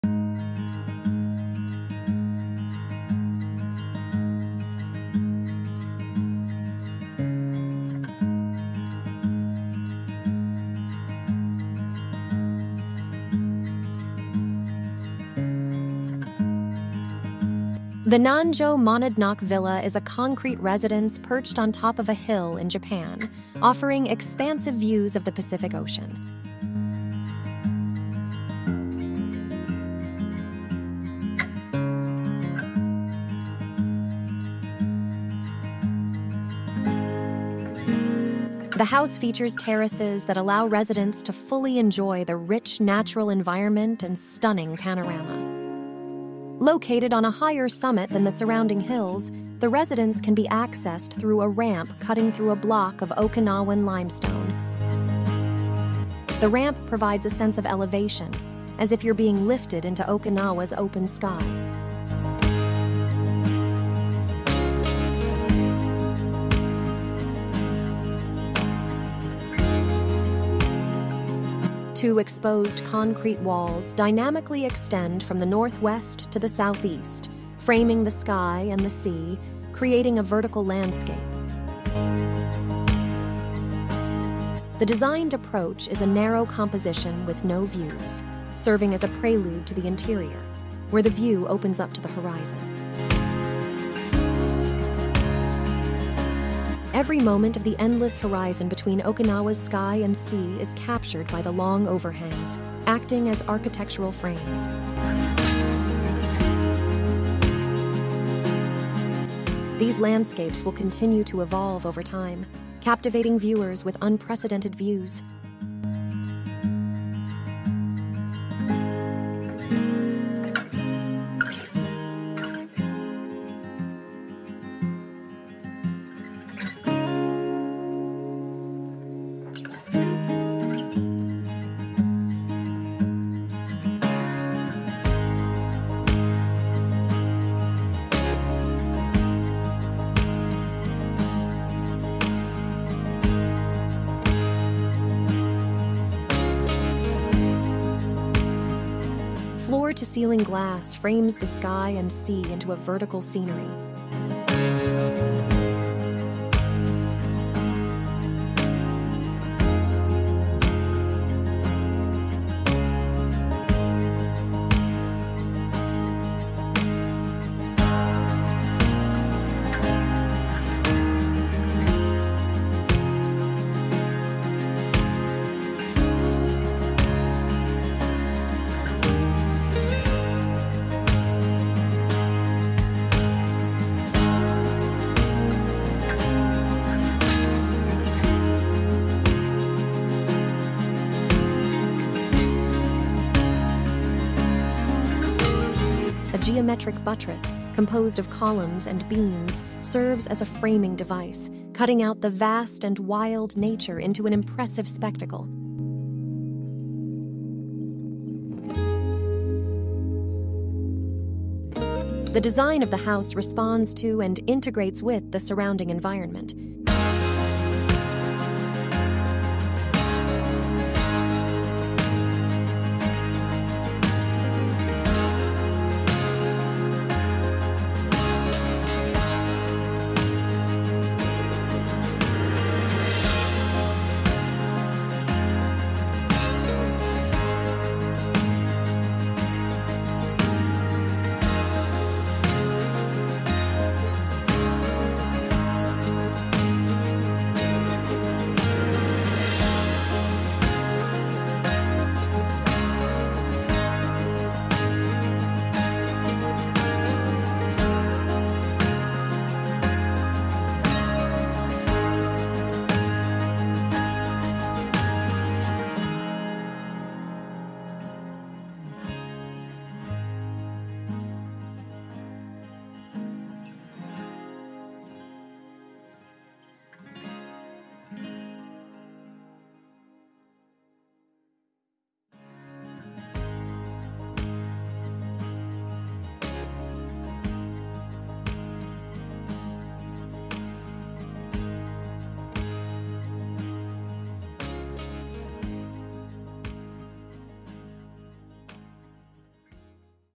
Concrete Hilltop House in Japan Overlooking the Pacific Ocean
The Nanjo Monadnock Villa is a concrete residence perched on top of a hill in Japan, offering expansive views of the Pacific Ocean. The house features terraces that allow residents to fully enjoy the rich natural environment and stunning panorama. Located on a higher summit than the surrounding hills, the residence can be accessed through a ramp cutting through a block of Okinawan limestone. The ramp provides a sense of elevation, as if you're being lifted into Okinawa's open sky.

Two exposed concrete walls dynamically extend from the northwest to the southeast, framing the sky and the sea, creating a vertical landscape. The designed approach is a narrow composition with no views, serving as a prelude to the interior, where the view opens up to the horizon.

Every moment of the endless horizon between Okinawa's sky and sea is captured by the long overhangs, acting as architectural frames. These landscapes will continue to evolve over time, captivating viewers with unprecedented views.

Floor-to-ceiling glass frames the sky and sea into a vertical scenery.

A geometric buttress, composed of columns and beams, serves as a framing device, cutting out the vast and wild nature into an impressive spectacle. The design of the house responds to and integrates with the surrounding environment.

Project info:

Name: Nanjo Villa Monadnock
Architect: Kidosaki Architects Studio
Principal in charge: Hirotaka Kidosaki
Project team: Kazuaki Mori
Location: Nanjo, Okinawa, Japan
Site area: 2,323.37 sqm
Building area: 216.68 sqm
Total floor area: 161.40 sqm
Structural engineer: Toyohito Shibamura
Development: Tasuku Miyagi
General contractor: Ki-kensetsu
Design year: 2017 – 2018
Construction year: 2019 – 2020
Lighting design: Yamada Shomei Lighting Co., Ltd.
Photos: 45g Photography